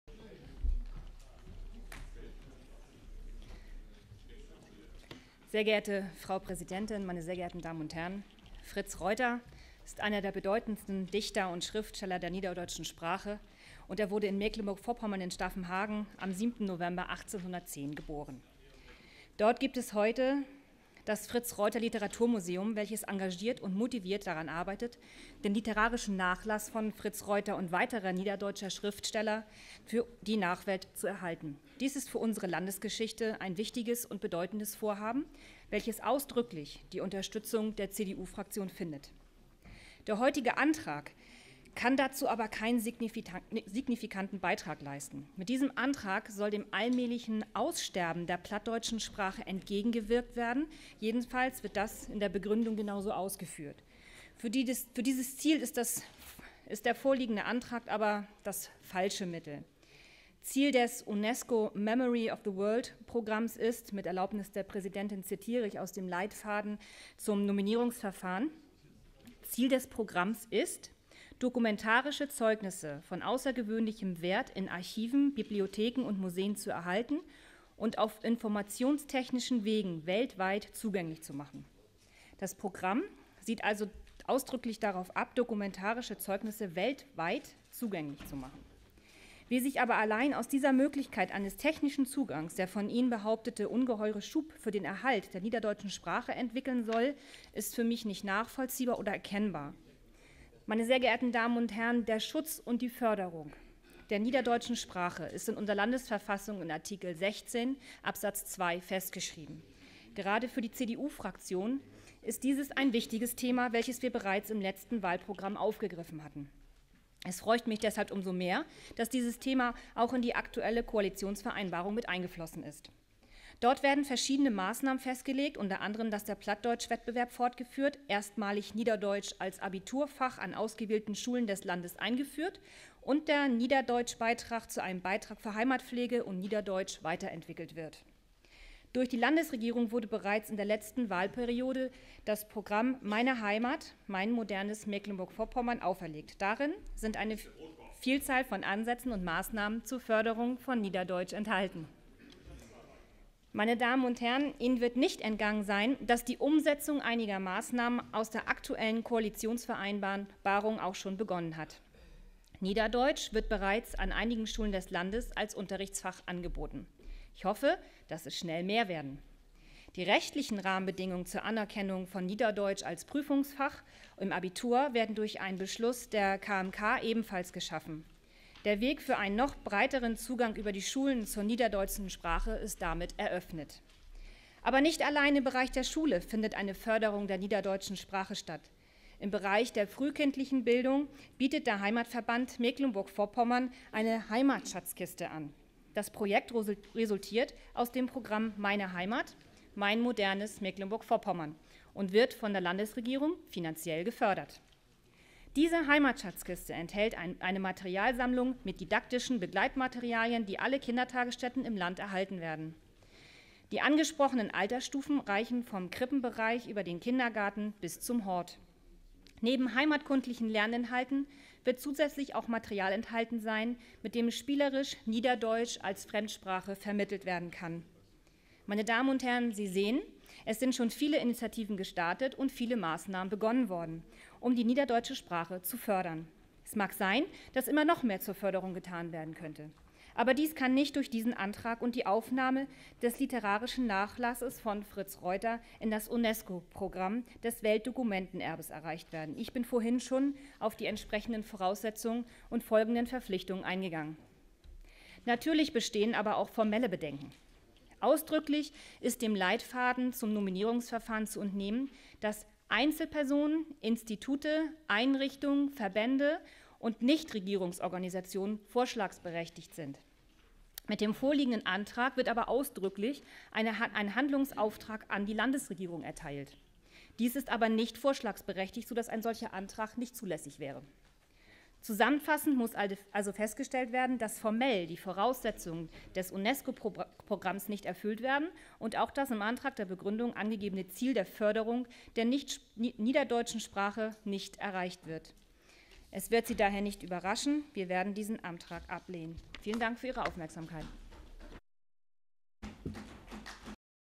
18.05.2017, Ann Christin von Allwörden, CDU-Fraktion, Landtag M-V, Fritz Reuter Weltdokumentenerbe
Die Kulturpolitische Sprecherin der CDU-Fraktion im Landtag Mecklenburg-Vorpommern, Ann Christin von Allwörden, spricht zum Antrag der AfD Fraktuion zum Thema "Aufnahme des literarischen Nachlasses von Fritz Reuter in die Liste des Weltdokumentenerbes der UNESCO".